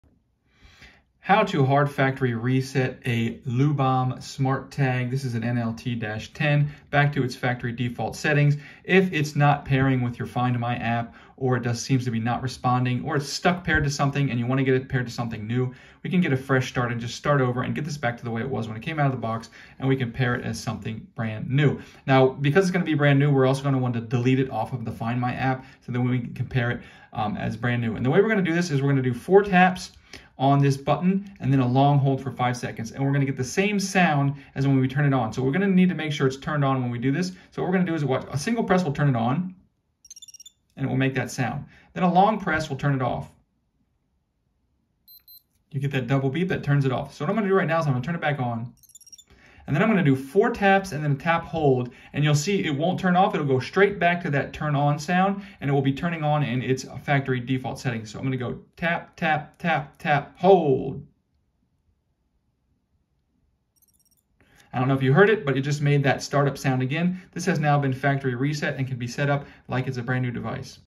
Factory Reset Luubom Smart Tag NLT 10
If your Luubom Smart Tag is not working or not pairing or connecting to the Find My app or your iPhone and you are troubleshooting, or you just want to delete the smart tag and set it up like new, you can perform this hard factory reset.